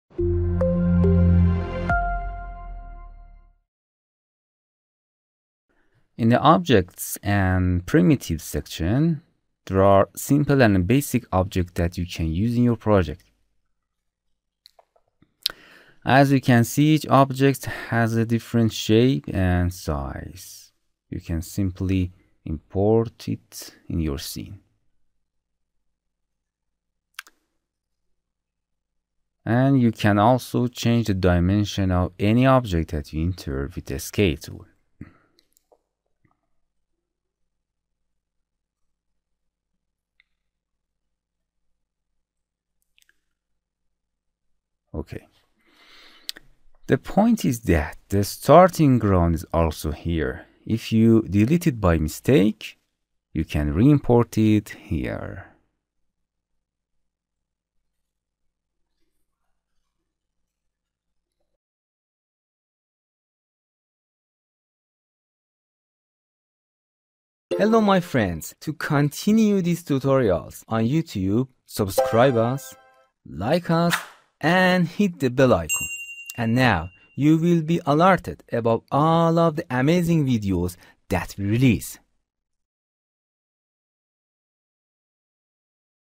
0023. primitives objects in twinmotion
In the objects and primitives section. There are simple and basic objects that you can use in your projects, each object has a different shape and size. The point is that the starting ground is also here, and if you delete it by mistake, you can re-enter it here. You can also change the dimensions of any object you enter with the scale tool.